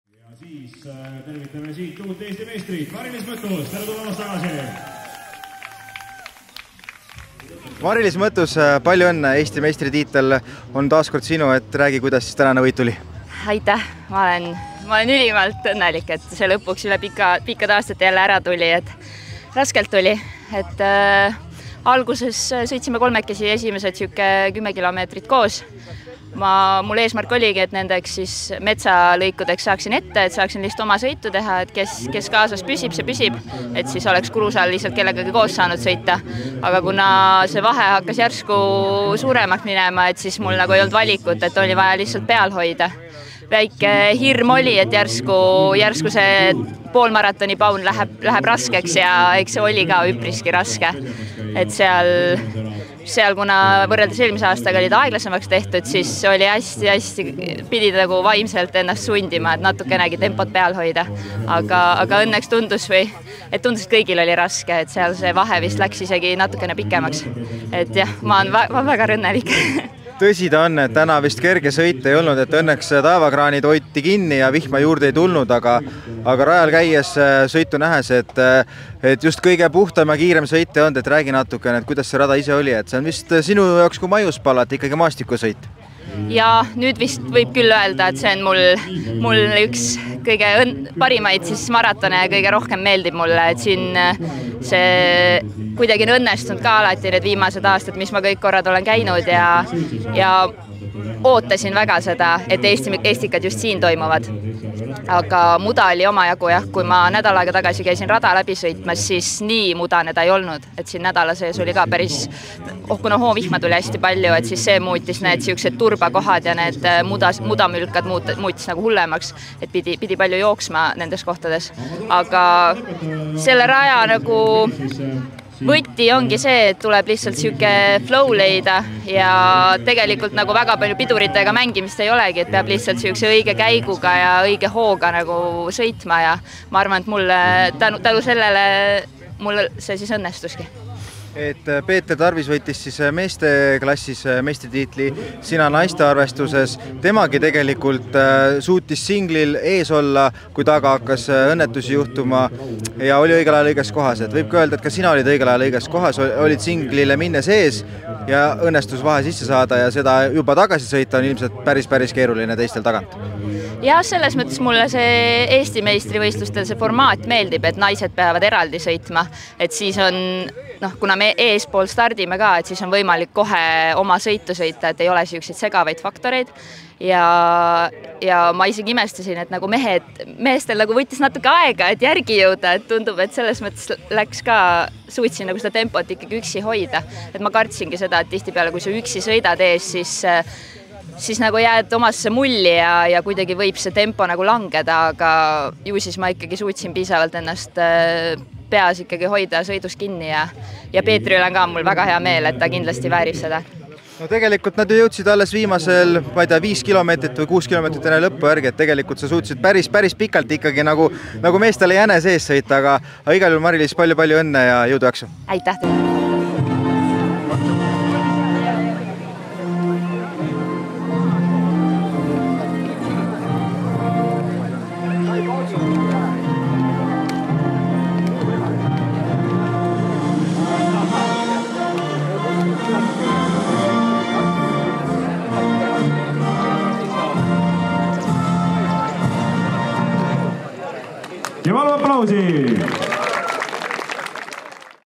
Mari-Liis Mõttus - Kose 7 järve rattamaratoni Naiste arvestuse võitja, XCM Eesti meister
Bosch Eesti Maastikurattasarja 10. hooaja kuuenda etapi, Kose 7 Järve rattamaratoni 66km pikkuse XXL-sõidu, mille raames selgitati välja ka käesoleva aasta Eesti meistrid maastikurattamaratonis, võitsid Mari-Liis Mõttus ja Peeter Tarvis. Harjumaal Paunküla ja Kose kandi radadel peetud erinevatest sõitudest võttis osa ligi 600 maastikuratturit.

Eile õhtul ja öösel sadanud vihma järel üsna libedaks muutunud raja esimesel tehnilisel osas Rõõsa singlitel hakkasid sinnani koos sõitnud naiste favoriitide vahed kärisema, kõige paremini sai keerutavatel singlitel hakkama Mari-Liis Mõttus (Haanja Rattaklubi), kes suutis 25km vahefinišiks jälitajate Janelle Uibokandi ja Greete Steinburgiga sisse sõita ligi 3 minutilise edu, kasvatades lõpujooneni seda veelgi ning lõpetas Eesti käesoleva aasta meistrina ajaga 3:07:45. Teisena lõpetas Greete Steinburg, aeg 3:12:34 ning EMV pronksi sai enda kaela Pelotoni esindaja Janelle Uibokand, aeg 3:18:32.

„Ma olen ülimalt õnnelik, et see üle pikkade aastate ära tuli, aga raskelt tuli. Eesmärk oli metsalõikudeks ette saada, et saaks oma sõitu teha, aga kuna vahe hakkas järsku suuremaks minema, siis tuli kiirust peal hoida. Hirm oli, et viimane paun läheb raskeks ja nii läkski, aga seal oli raske ilmselt kõigil,“ sõnas naiste sõidu võitja ja värske Eesti meister Mari-Liis Mõttus.

Meeste sõidus sai Rõõsa singlitel suuremast juhtgrupist nelik, ka 25km vaheajapunktis oldi neljakesi vaid 2 sekundi sees, samas edestades taganttulijaid juba üle pooleteise minutiga. Sõidu teises pooles, 40km peal, hakkas juhtgrupp lõplikult lagunema, eest sai ära Peeter Tarvis (KoMo), kes jätkuvalt Gert Jõeäärega vahet suurendades lõpetas 66km pikkuse sõidu Eesti meistrina ajaga 2:41:14, edestades veidi üle 3 minutiga EMV hõbeda pälvinud Jõeäärt (CFC). Kolmandaks sõitis ennast Pelotoni esindaja Gert Kivistik, tema lõpuajaks märgiti 2:45:13!

“Tükk aega on möödas viimasest maratonivõidust üldse, aga eestikatel suudan ennast realiseerida tavaliselt. Paar meest olid puudu ka – Pruus, Vaidem, Pajur - aga täna olin mina parim! Konkurendid tegid singli peal sõiduvigasi, sain esimesena singlilt välja, tagant oli tühi, hoidsin peal ja nii lõpuni välja,” sõnas värske Eesti meister Peeter Tarvis.

50km põhisõidu võitsid narvakas Artur Patrenkin ja Beate Kogvere Lätist, 17km pikkusel poolmaratonil olid noorte seas parimad Magnus Normak ja Berit Grünberg.
Lisaks toimusid ka lastesõidud, mille erinevatel sõitudel osales ja lustis 78 mudilast.

Tiimide arvestuses võidutsesid Spordipartner Racing Girls naiskondade- ning Veloplusi tiim meeskondade arvestuses.

Bosch Eesti Maastikurattasarja hooaja seitsmes- ja ka finaaletapp sõidetakse 31. augustil taas Harjumaal, kui Aegviidus antakse start Eesti vanimale, juba 27ndat korda toimuvale Tallinna Rattamaratonile.